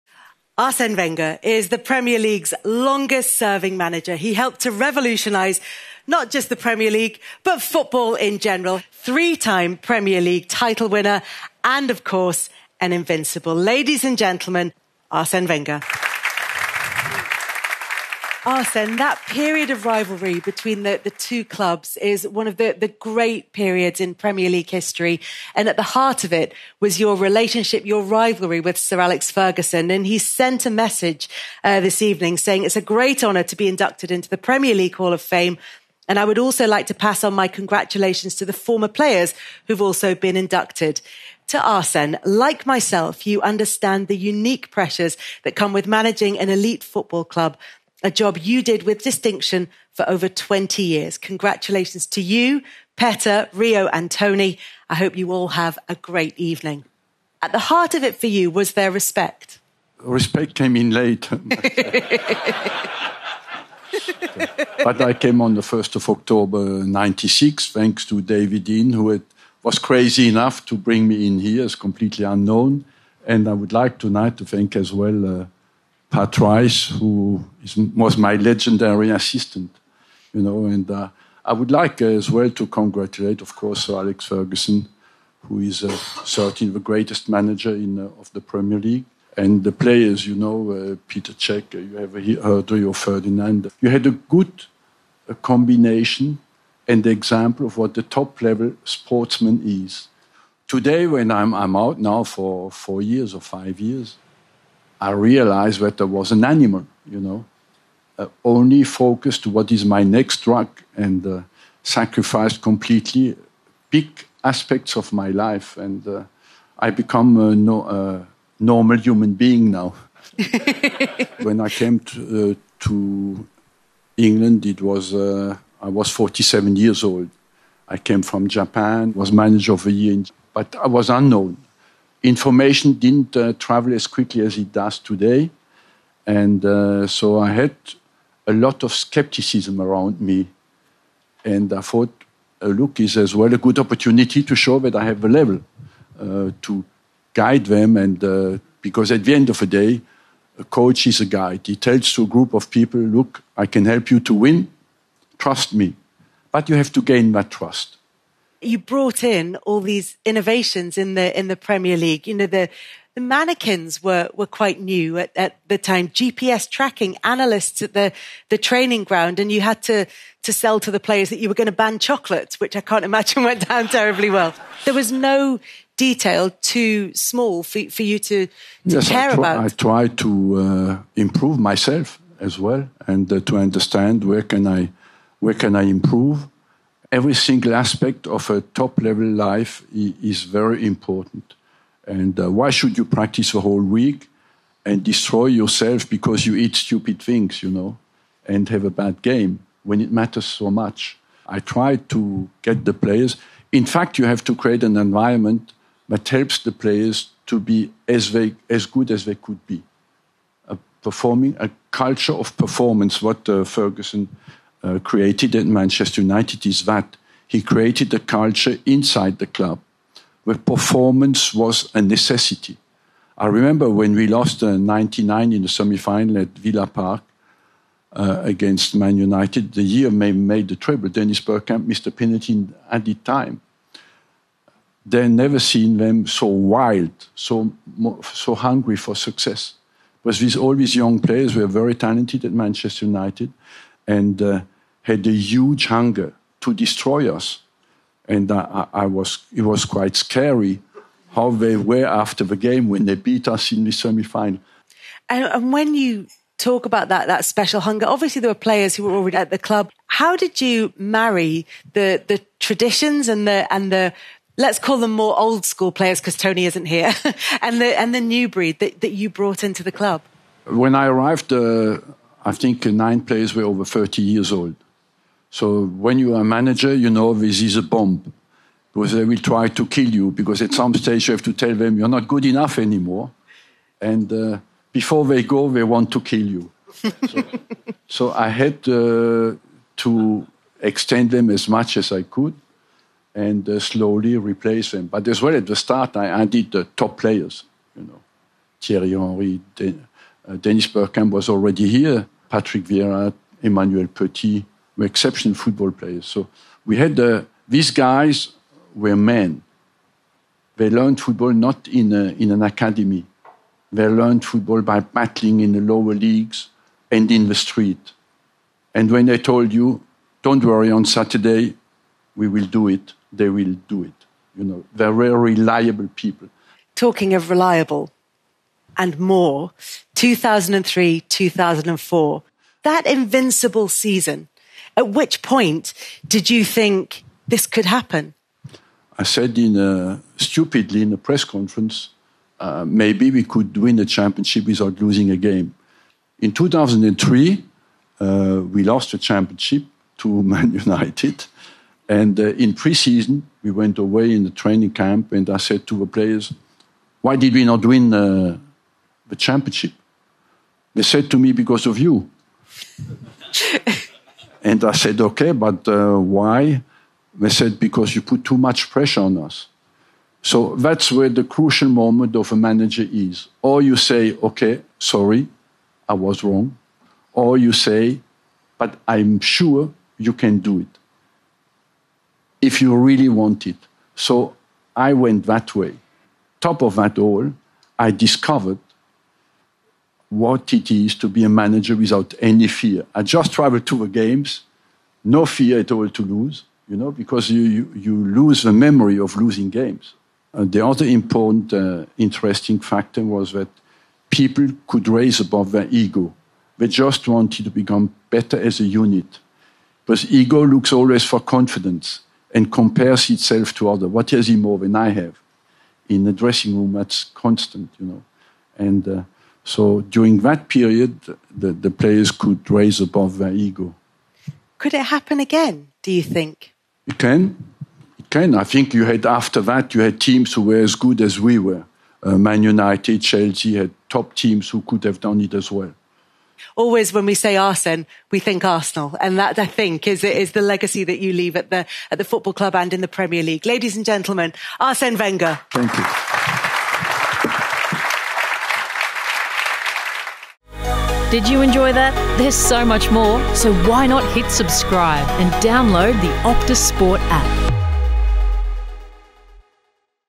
'Respect came later' | Wenger and Sir Alex Ferguson's HISTORIC rivalry
Kelly Cates sits down with Arsene Wenger at the Premier League Hall of Fame awards for an in-depth chat about his rivalry with Sir Alex Ferguson.